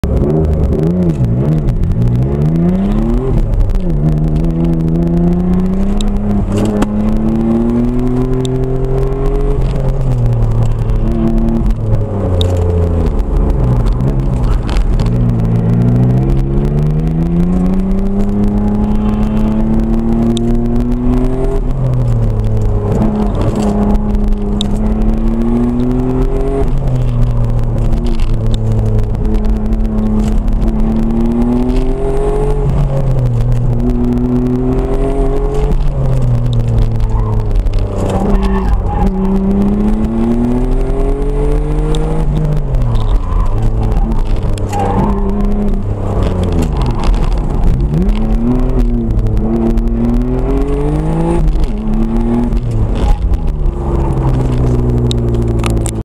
Bremerton Raceway 3000gt VR-4 final run, 6-10-12
Ran a 49.7xx, had a bangin 1-2 shift at the end.

Stock motor, stock turbos, E-cutout, upgraded sidemounts, upgraded radiator, street tires, 14" brakes